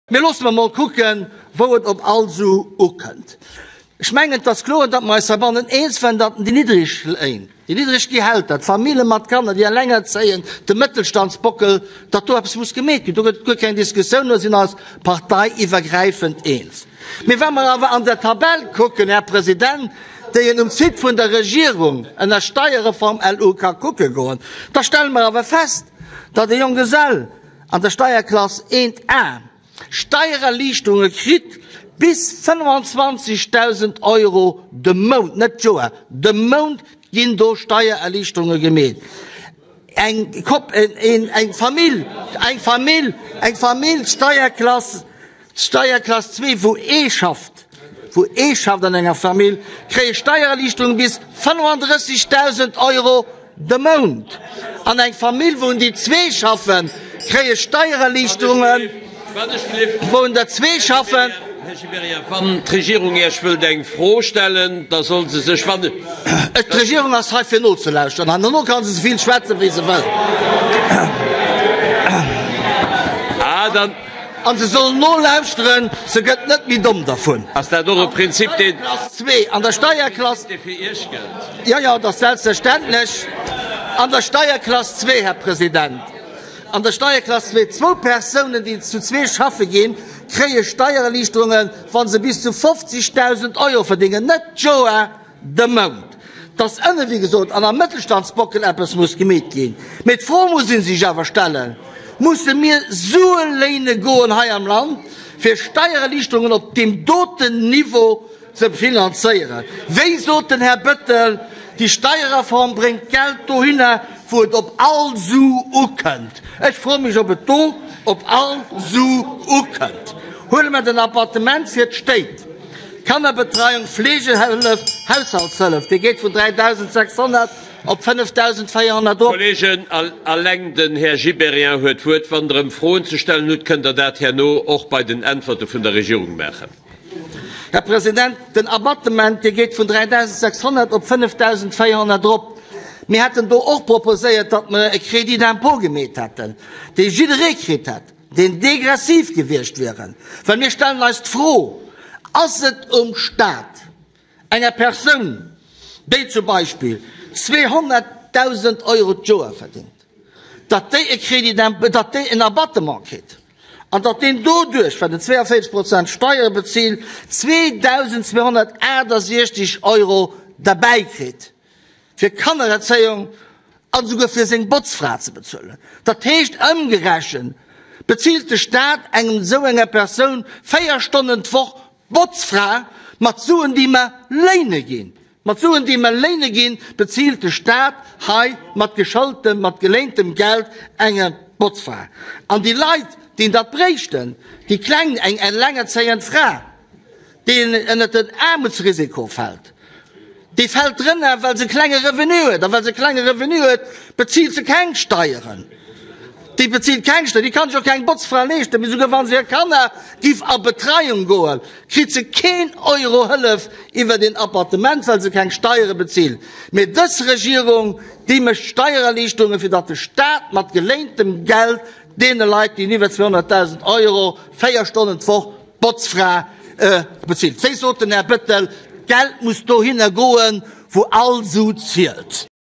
Do wou all Su zielt
De Gast Gibéryen iwwer Lag vun der Natioun, Steierreform an den Abattement fir d'Botzfra